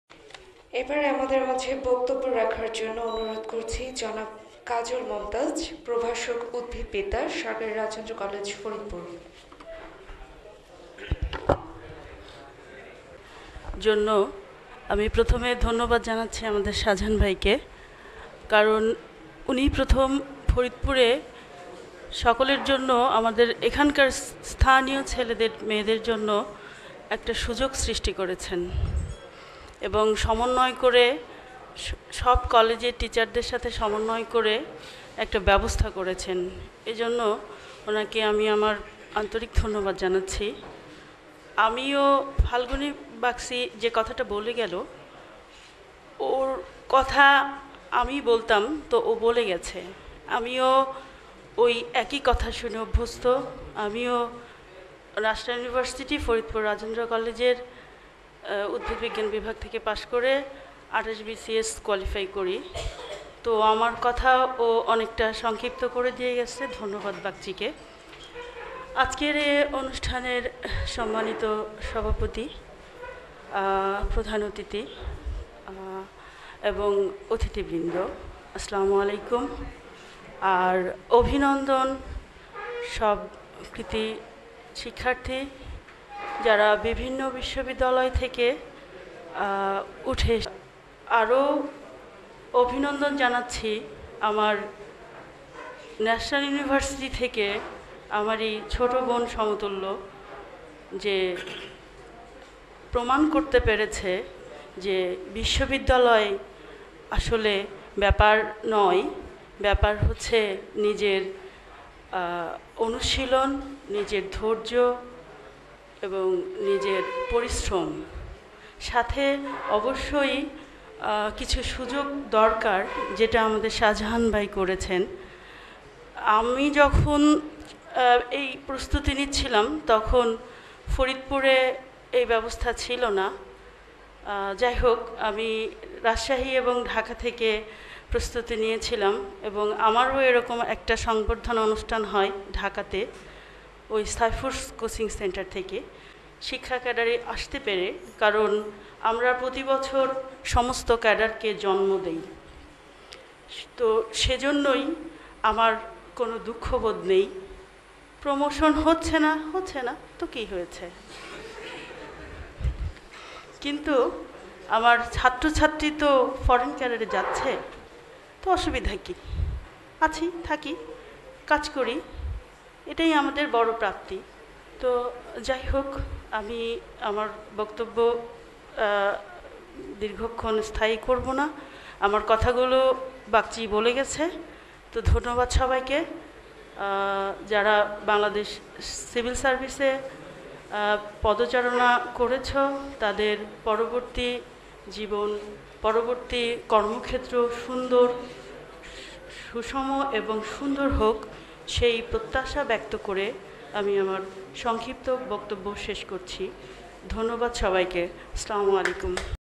Warm Reception Speech by Kazal Momotaj, Lecturer, Botany, Govt Rajendra College, Faridpur o
কৃতী শিক্ষার্থী সংবর্ধনা অনুষ্ঠানে জনাব কাজল মমতাজ, প্রভাষক, উদ্ভিদবিদ্যা বিভাগ, সরকারি রাজেন্দ্র কলেজ, ফরিদপুর। স্থানঃ বারবাডোজ রেস্টুরেন্ট, ফরিদপুর।